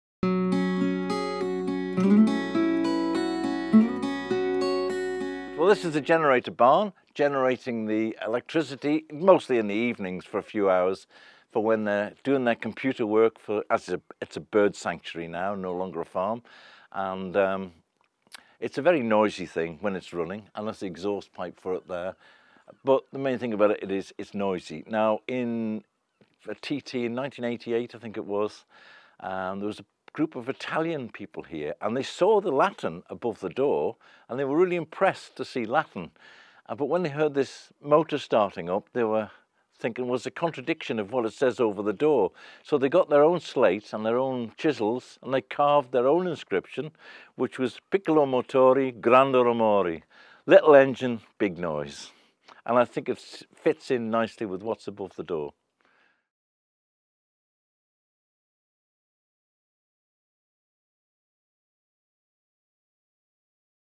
Italian graffiti on the Calf of Man
Why is there an Italian inscription outside the engine house on the Calf of Man?
The surprising story of T.T. visitors not so long ago reacting to the 1878 sign on George Carey's farmhouse...